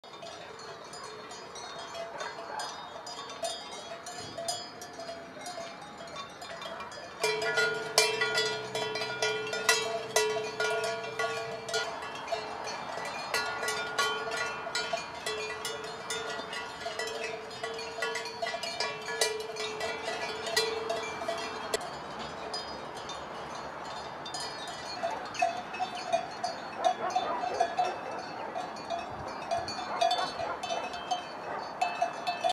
Pastorella
A la cueretablanca (motacillaalba) al Berguedà i altres indrets se l’anomena pastorella. És present a molts ambients diferents, però també és un ocell molt associat als ramats. Menja els invertebrats que atrau o aixeca el bestiar quan pastura. També anomenada enganyapastors.